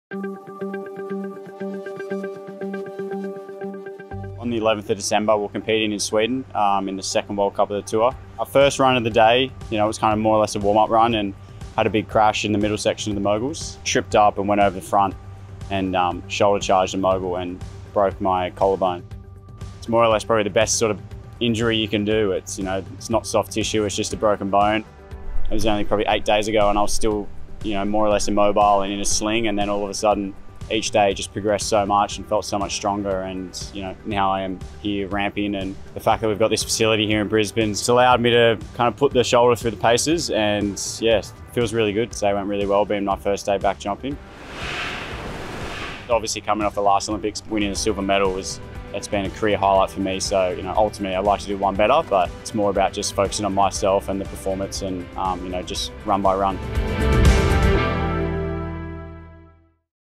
Matt Graham recovers from injury & prepars for Beijing2022 at Brisbane's Olympic training facility
Moguls skier Matt Graham recovered from a snapped collarbone at the Geoff Henke Winter Olympic Training Centre in Brisbane, one month out from the Beijing2022 Olympic Winter Games.